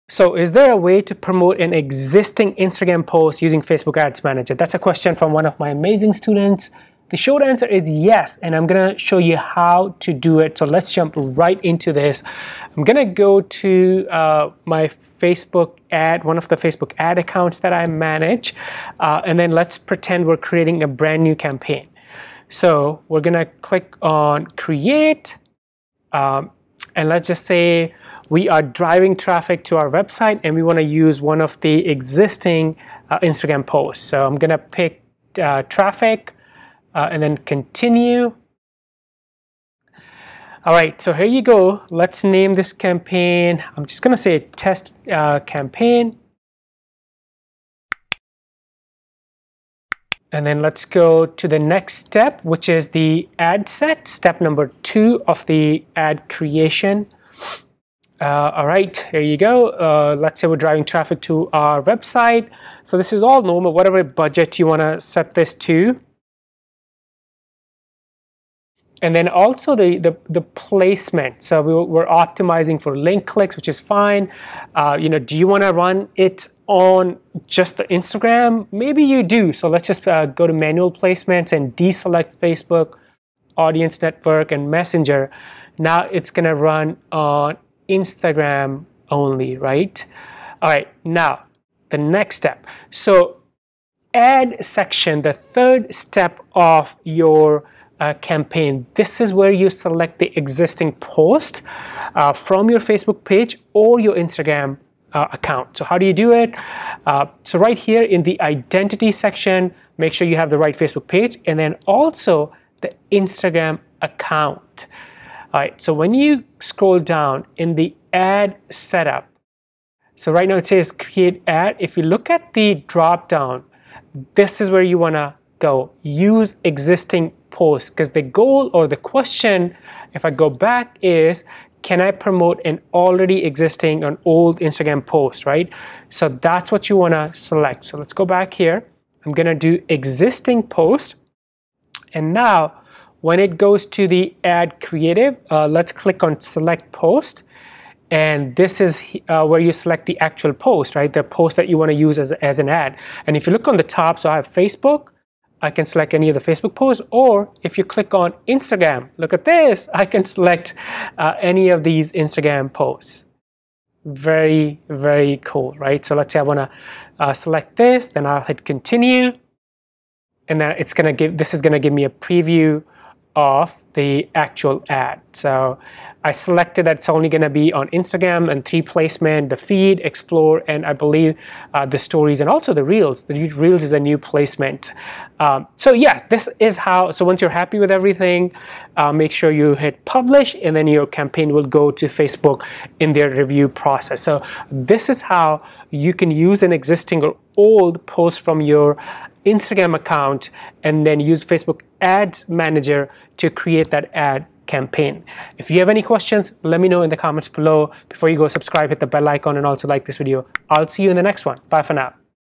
How To Promote Instagram Post With Facebook Ads
How To Promote Instagram Post With Facebook Ads. When we create Facebook ads inside Facebook Ads manager, we can use an existing or old Instagram post as an Ad creative. I'll share all the steps in this video.